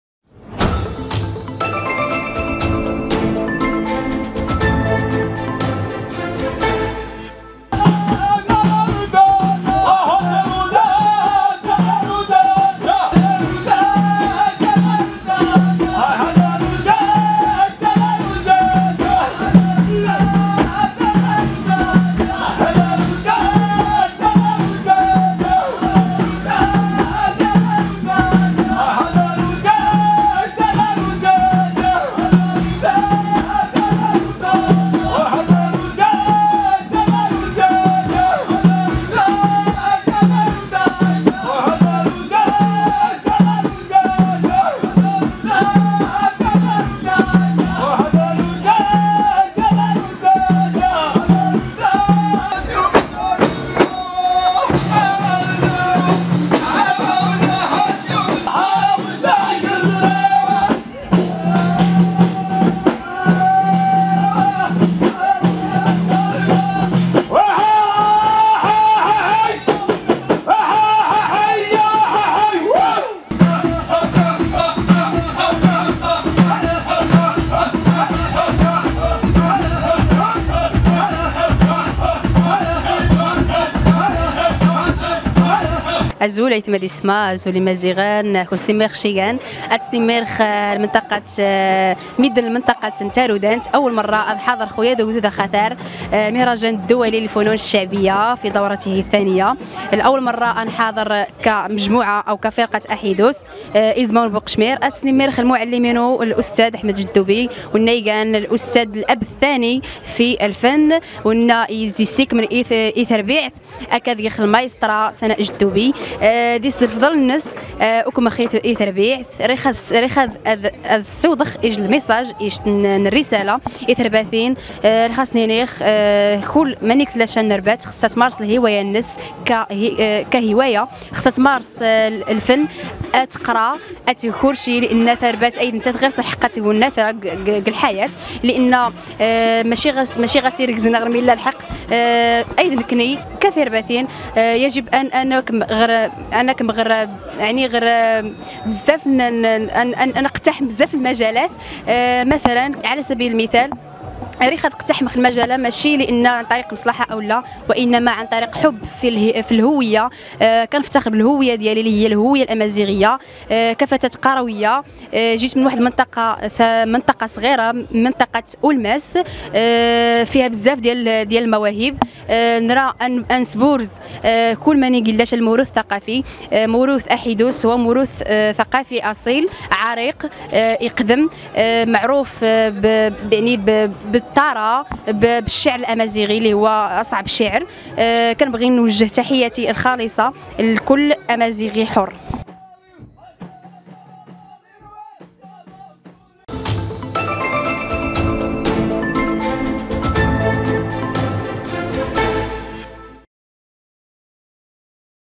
سناء أصغر مَايْسترَا تقود فرقة أحيدوس بالمغرب
تصريح الموهبة الشابة سناء، المنحدرة من منطقة أولماس بصفتها أصغر مايسترا تقود فرقة أحيدوس بالمغرب